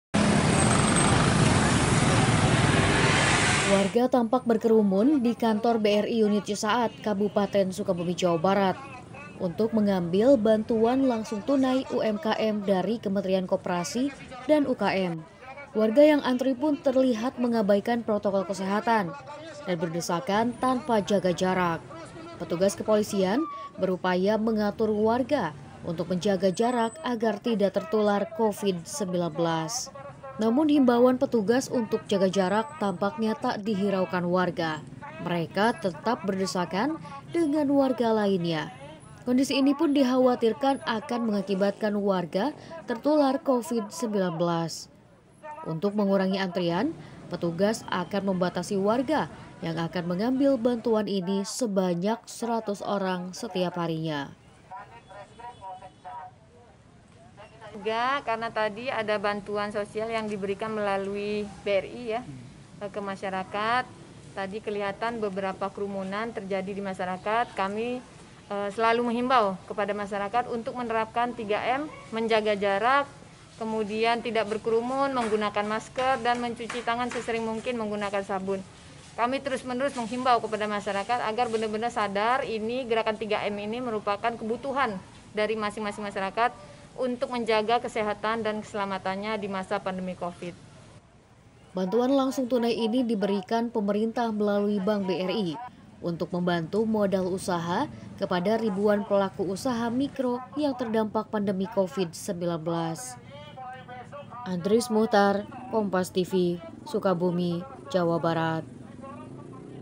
Antre Bantuan UMKM, Polisi Himbau Warga Patuh Prokes 3M
SUKABUMI, KOMPAS.TV - Warga tampak berkerumun di Kantor BRI Unit Cisaat, Kabupaten Sukabumi Jawa Barat, untuk mengambil bantuan langsung tunai UMKMdari Kementerian Koperasi dan UKM. Warga yang antre pun terlihat mengabaikan protokol kesehatan dan berdesakan tanpa jaga jarak. Petugas Kepolisian berupaya mengatur warga untuk menjaga jarak agar tidak tertular Covid-19. Namun himbauan petugas untuk jaga jarak, tampaknya tak dihiraukan warga, mereka tetap berdesakan dengan warga lainnya. Kondisi ini dikhawatirkan mengakibatkan warga tertular Covid-19.

Untuk mengurangi antrean, petugas akan membatasi warga yang akan mengambil bantuan ini, sebanyak 100 orang setiap hari. Bantuan langsung tunai ini diberikan pemerintah melalui Bank BRI, untuk membantu modal usaha kepada ribuan pelaku usaha mikro yang terdampak Pandemi Covid-19.,